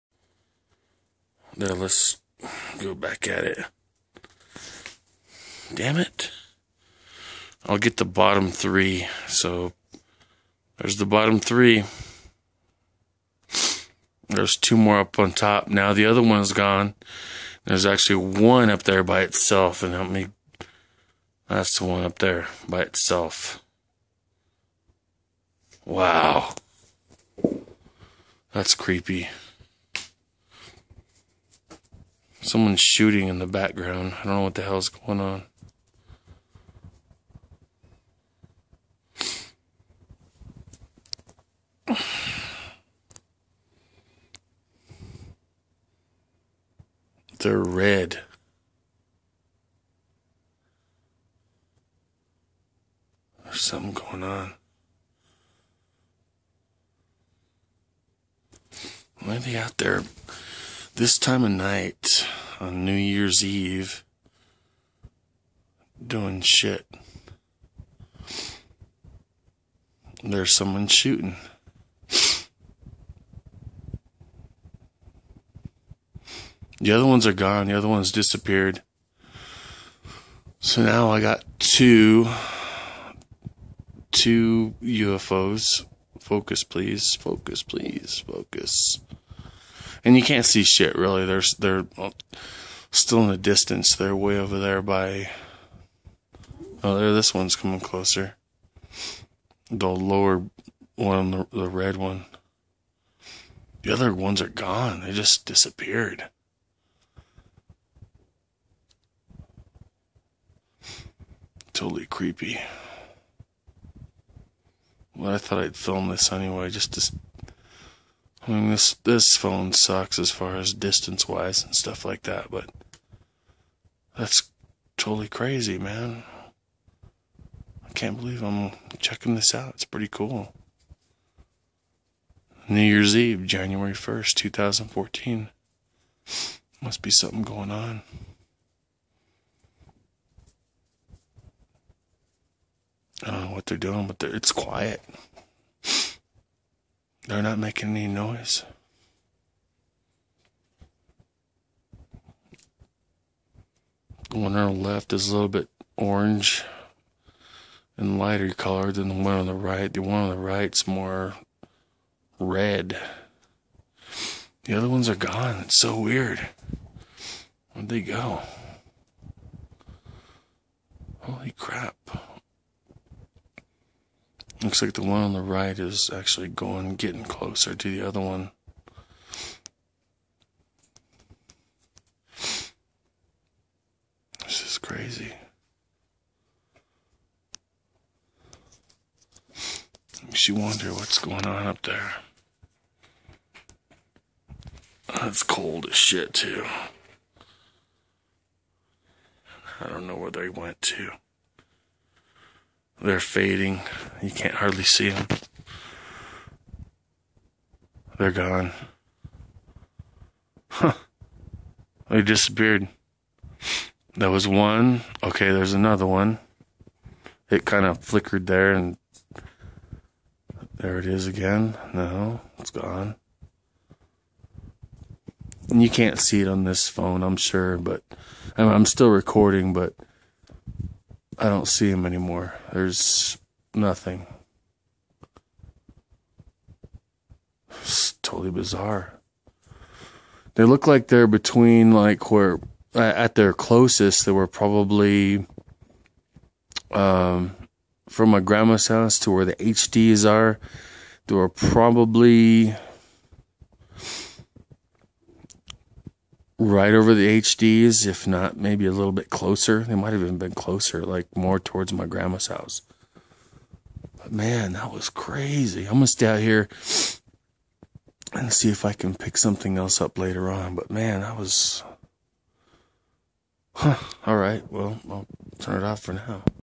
UFO sighting outside of Bayfield Co
This is a short video of UFO activity in southwestern Colorado. January 1st 2014.